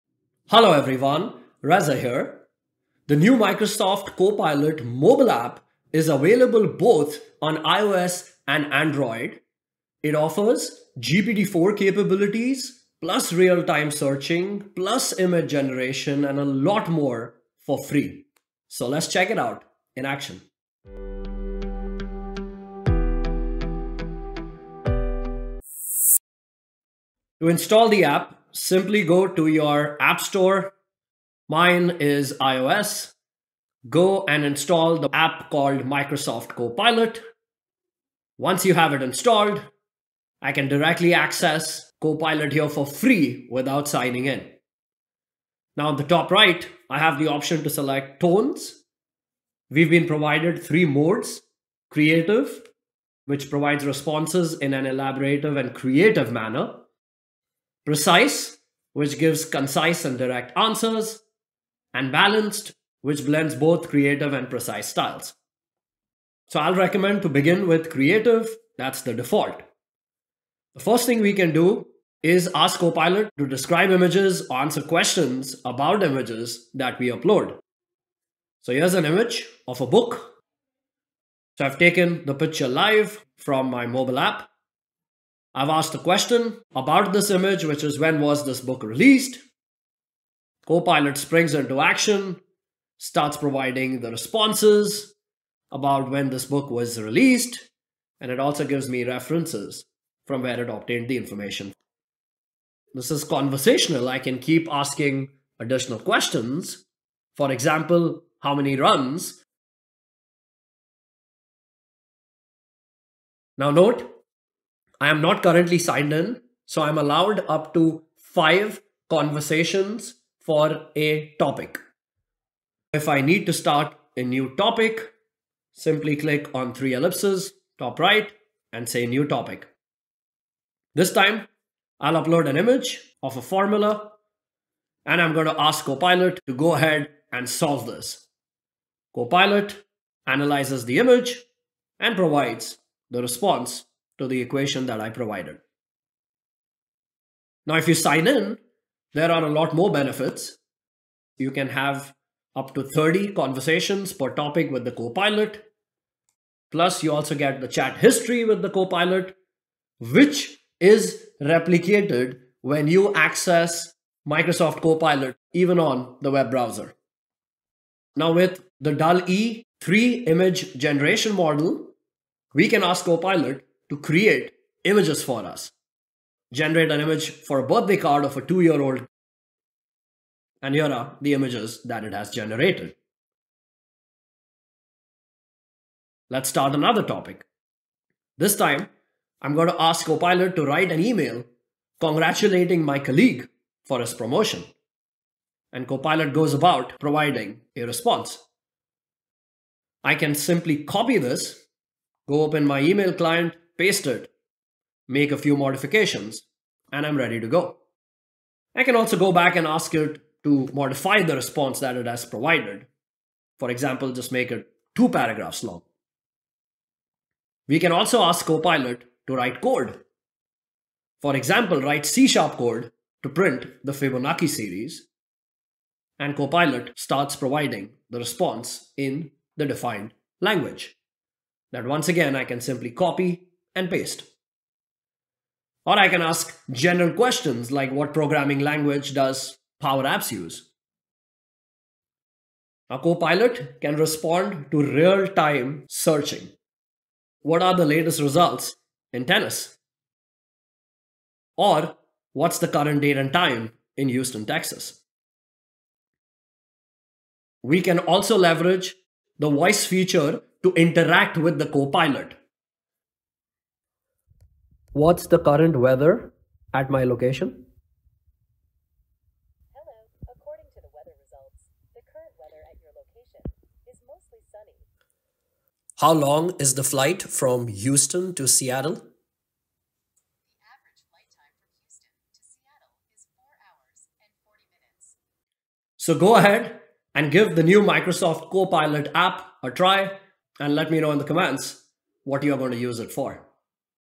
Meet Microsoft Copilot App: Your Ultimate FREE AI Assistant!
Microsoft launched the Copilot App for Mobile, designed to be your ultimate AI companion for Android and iOS, completely FREE of charge.

🌐 Free access to OpenAI’s powerful GPT-4 model
🎨 Unleash your creativity with DALL-E 3 image generation.
💻 Write Content and Simplify coding tasks with Copilot's coding assistance.
🔍 Experience real-time searching
What can the App do?
🖼️ Generate images with words
✍️ Write content, ask questions, write code, describe images, and more!
🔉 Utilize GPT-4, Dall-E 3, and voice chat everywhere – all for FREE!

Table of contents:
00:00 - Introduction to Microsoft Copilot Mobile App
00:27 - Install Microsoft Copilot Mobile App (iOS or Android)
00:45 - AI conversation styles - Creative, Precise and Balanced
01:10 - Use Copilot to describe images or ask questions about Images (Vision)
02:27 - Sign in with your Microsoft Account (FREE)
02:47 - Use AI to Create Images with Words (DALL.E 3)
03:08 - Use AI to write content in seconds
03:37 - Use AI to write code
04:02 - Real time searching with Microsoft Copilot App
04:17 - Use Voice Chat with new Copilot Mobile App
04:45 - Closing